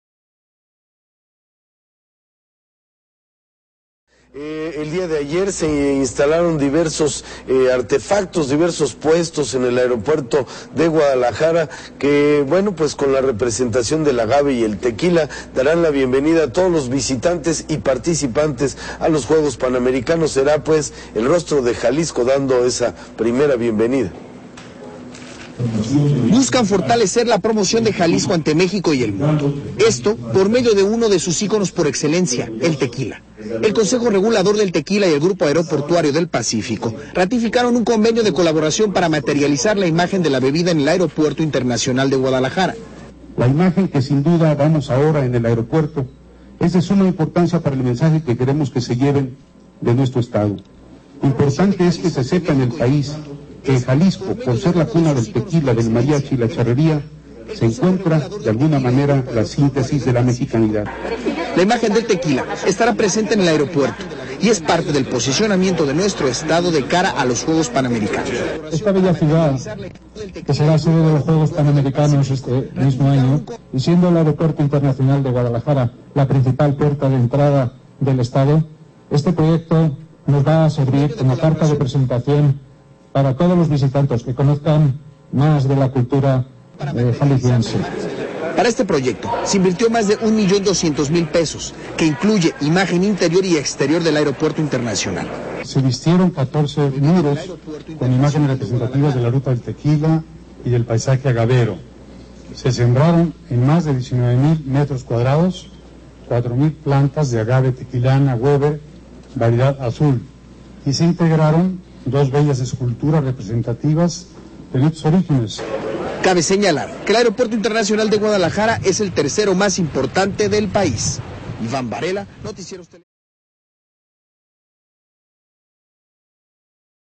Tequila en el aeropuerto de Guadalajara
De cara a los Juegos Panamericanos y con el objetivo de dar una mejor imagen de Jalisco, el CRT, GAP y la Secretaría de Turismo unieron esfuerzos para impactar gratamente a los visitantes que llegan al Aeropuerto Internacional de Guadalajara con la "Imagen del Tequila"